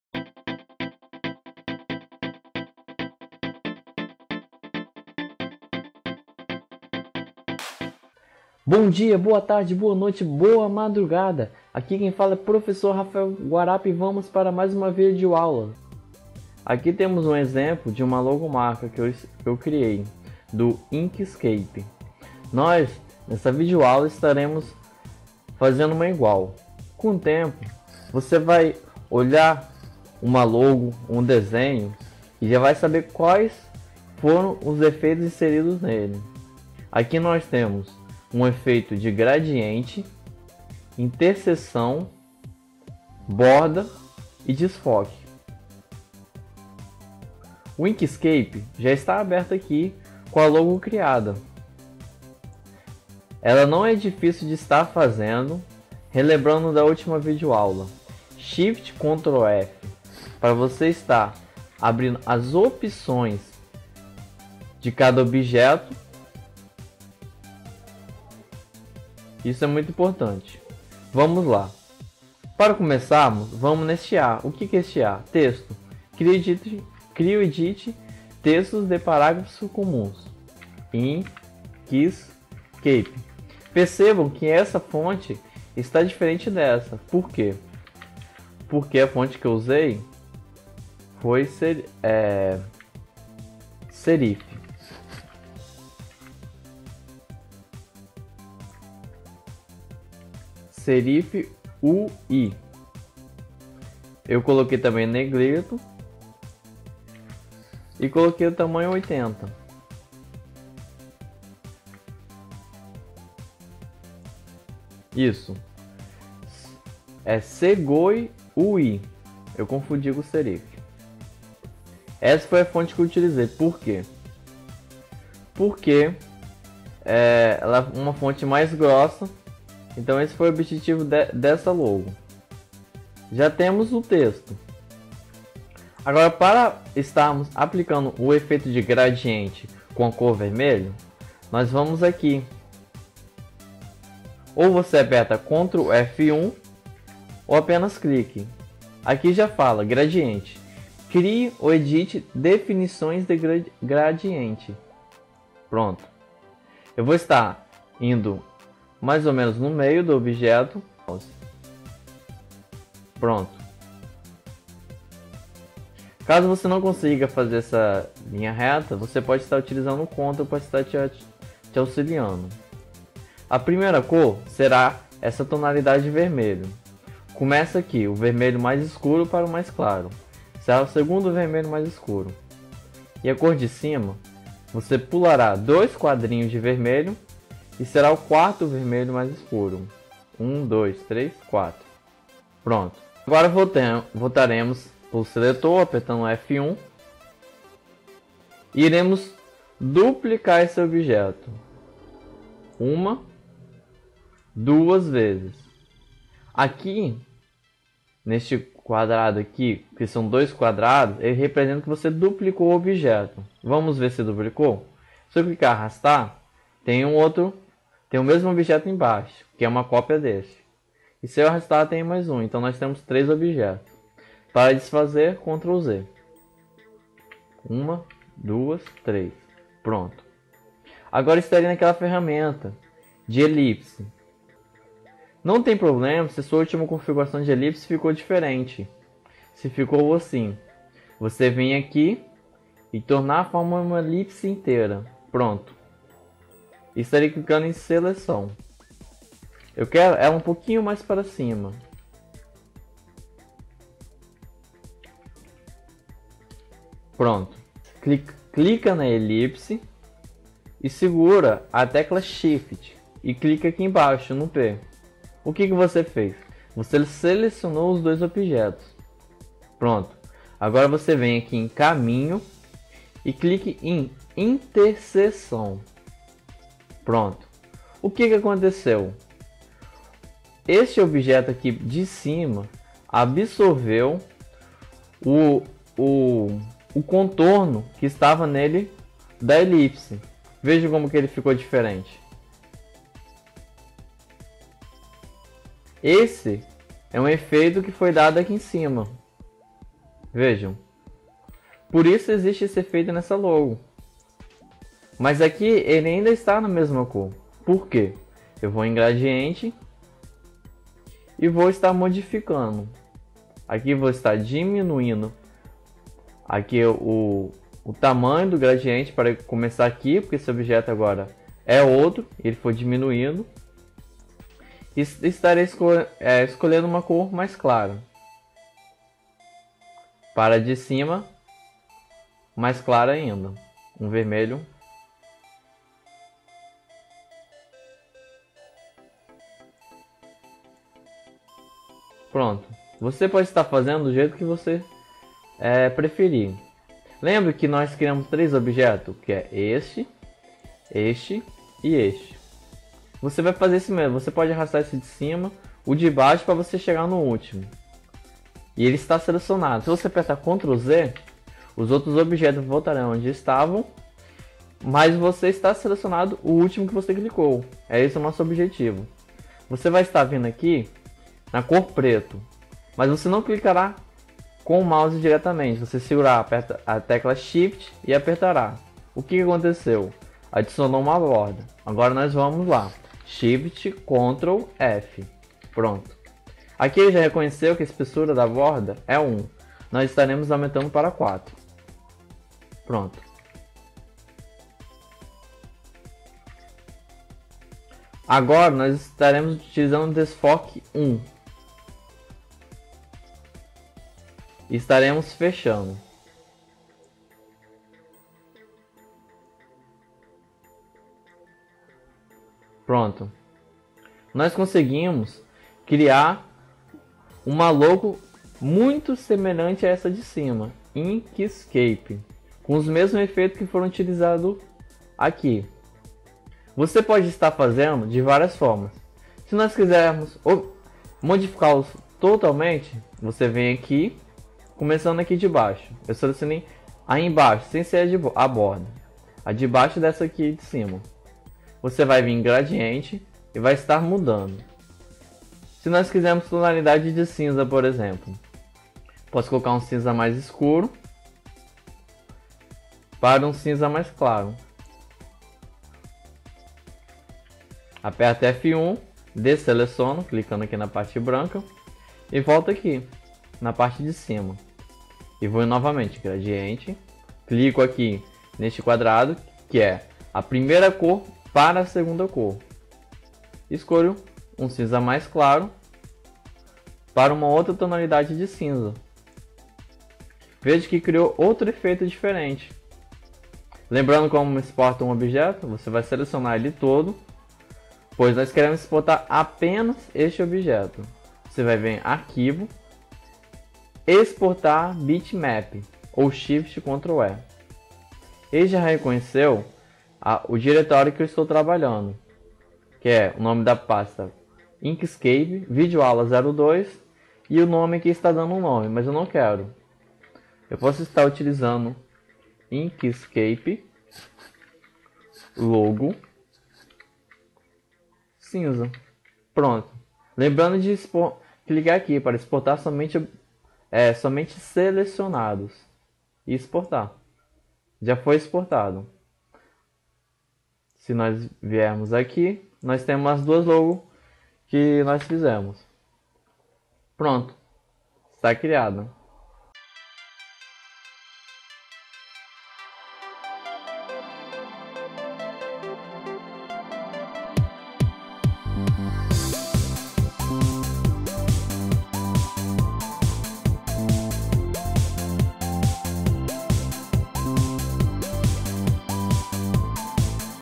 Curso de Design Inkscape - 02 - criando uma logo marca
Aprenda como criar, desenhar e editar uma Logo marcar no Inkscape.